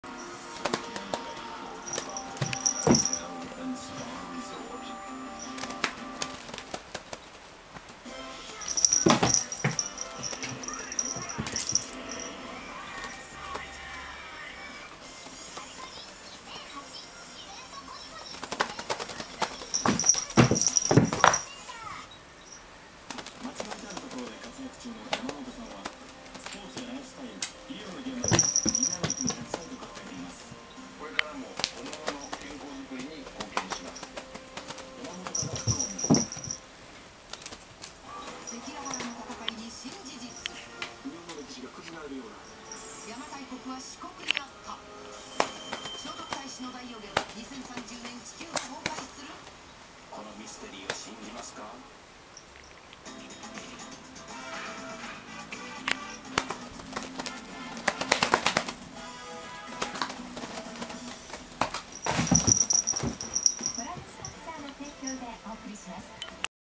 Attack of the Nimai
Nimai jumps onscreen to attack cat toy, meanders offscreen, repeat.